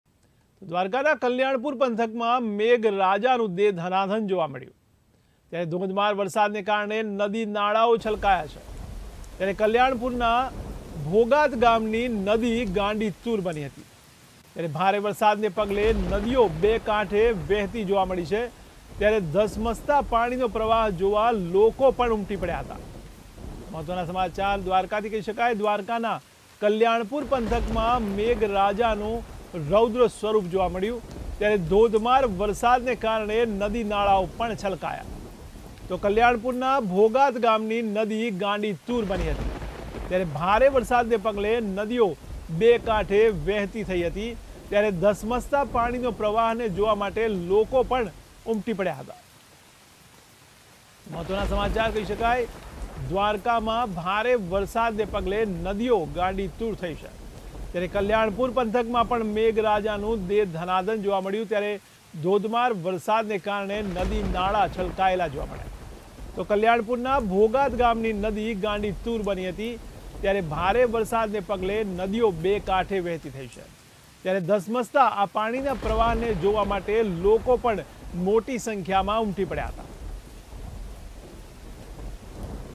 દ્વારકા: કલ્યાણપુર પંથકમાં મેઘરાજાનું દે-ધનધાન
ધોધમાર વરસાદના કારણે નદી-નાળા છલકાયા
કલ્યાણપુરના ભોગાત ગામની નદી બની ગાંડીતૂર
ભારે વરસાદને પગલે નદીઓ બે-કાંઠે વહેતી થઈ
ધસમસતા પાણીનો પ્રવાહ જોવા લોકો ઉમટયા